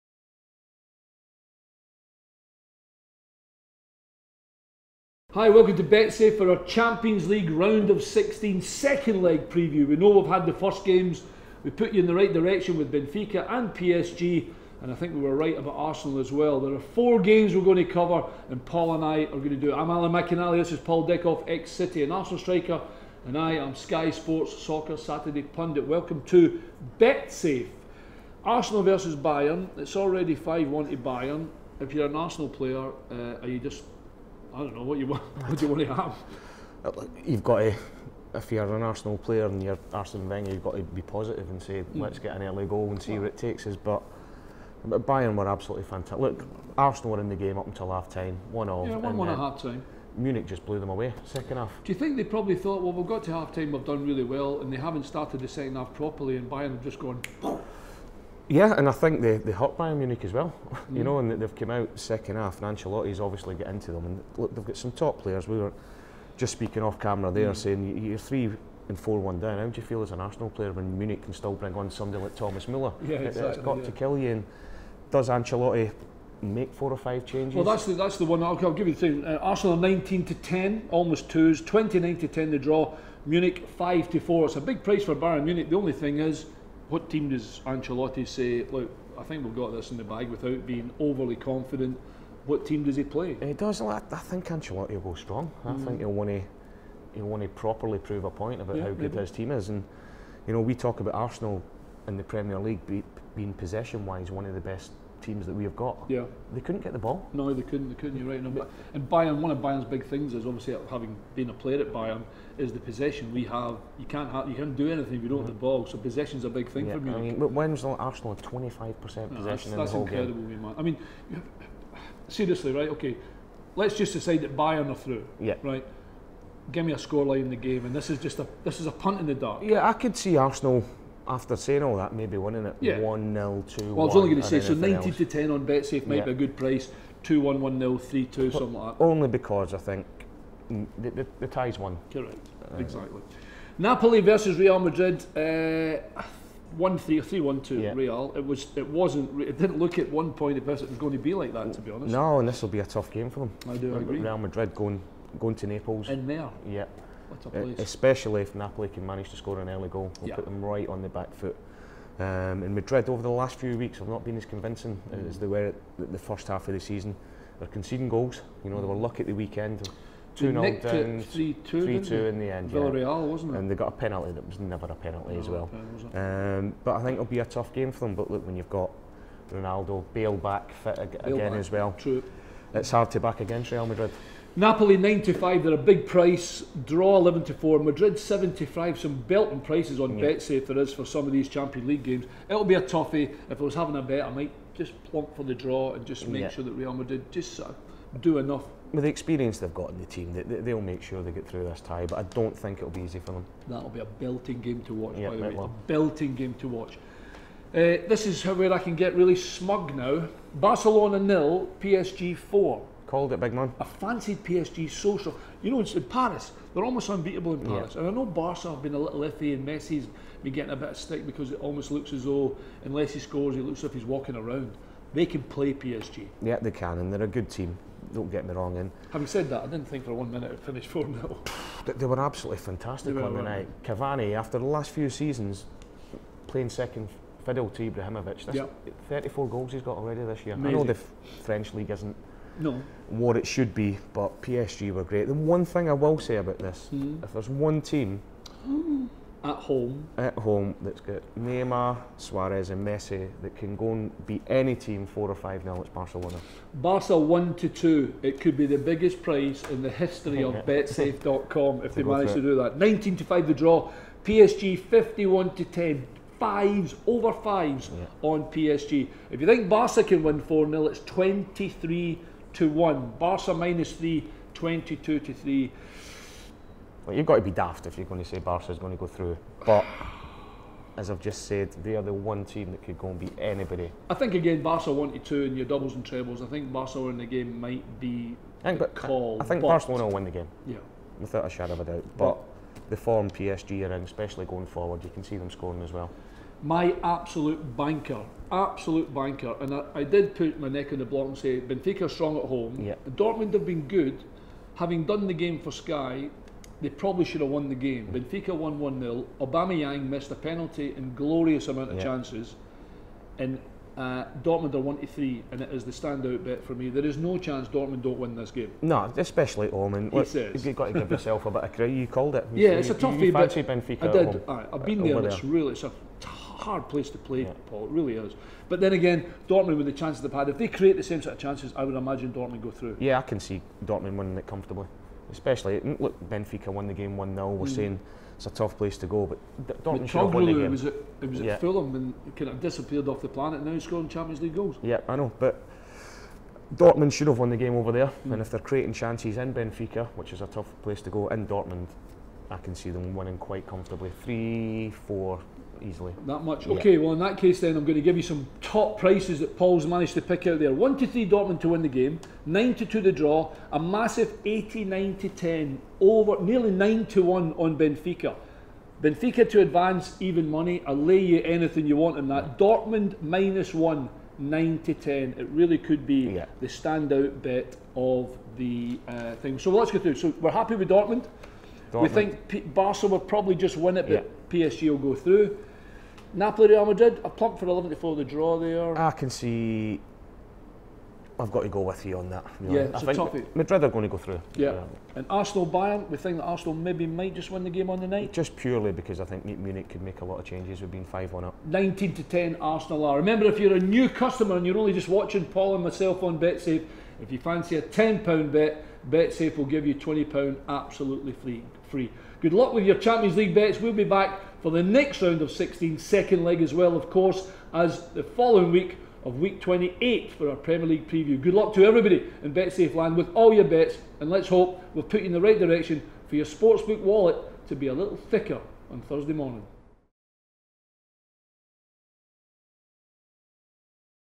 Champions League Last 16 second leg - 7/8 March
Alan McInally and Paul Dickov give their expert tips ahead of the second leg last 16 matches on the 7/8 March.